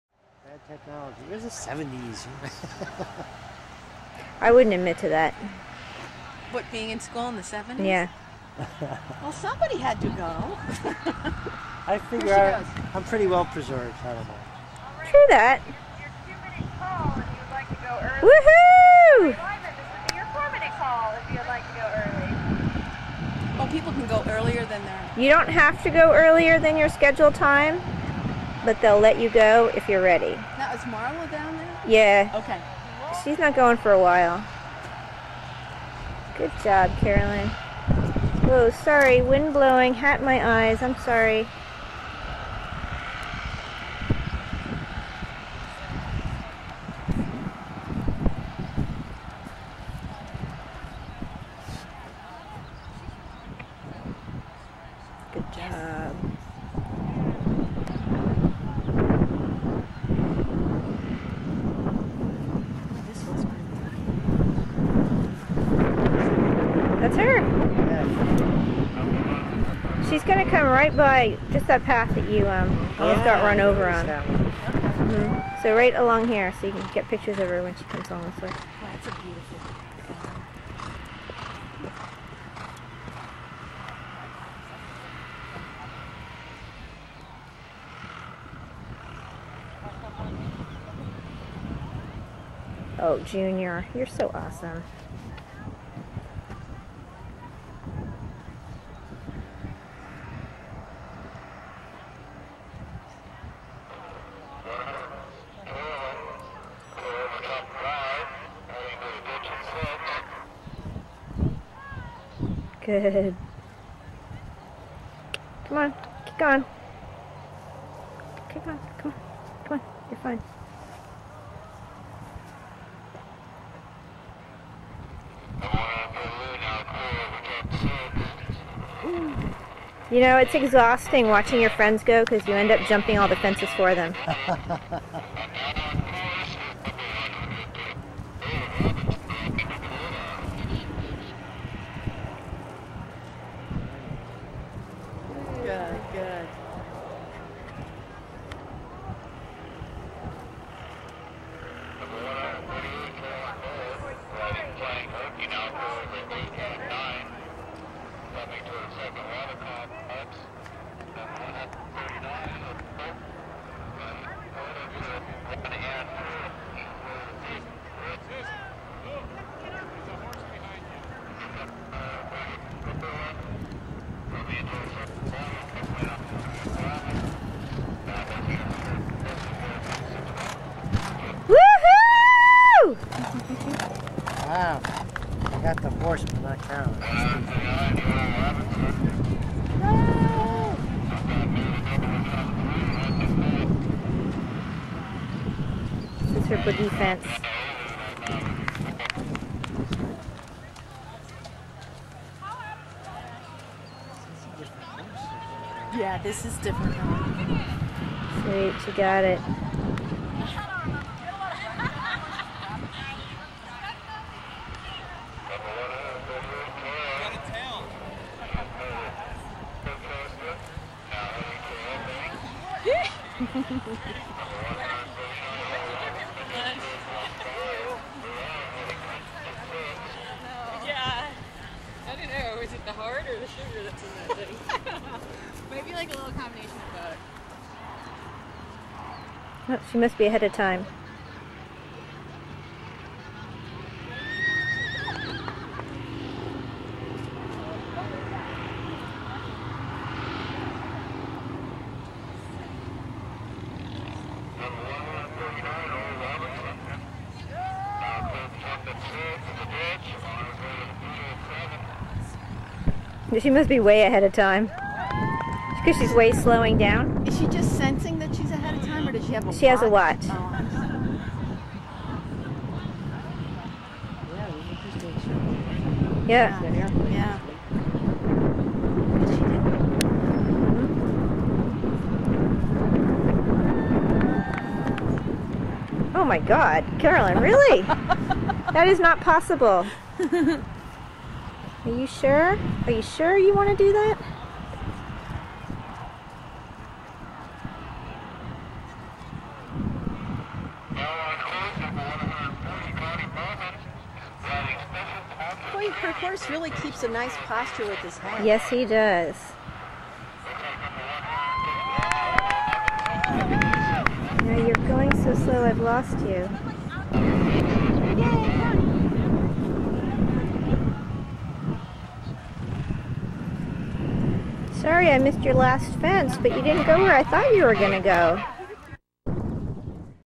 Junior_XC_2011.06
Carolyn riding Junior cross country at Shepherd Ranch, June 2011